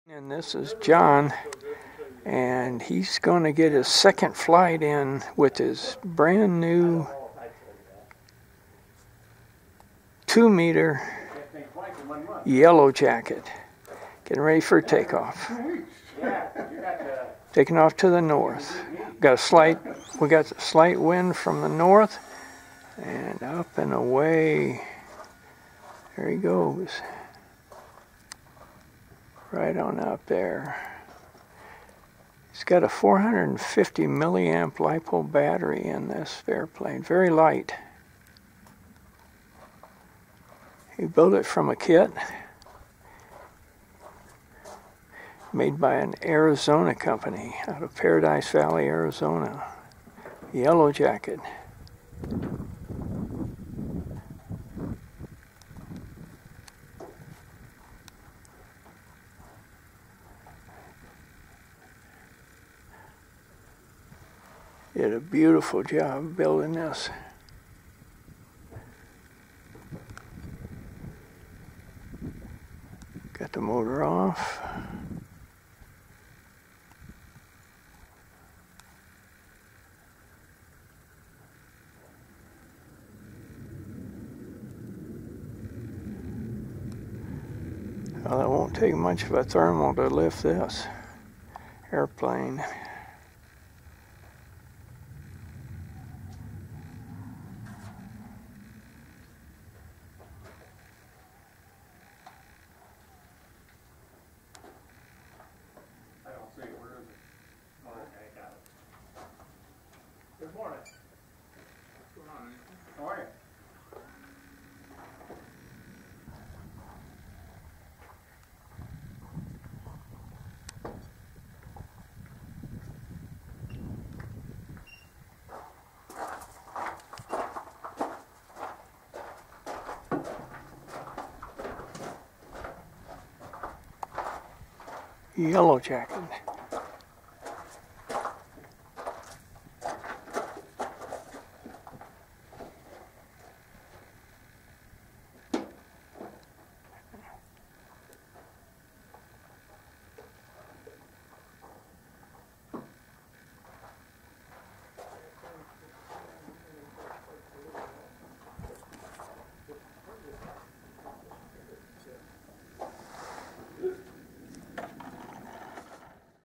Two Meter Yellow Jacket glider 6-27-19.
The kit for this beautiful light-weight glider is from Sonoran Laser Art.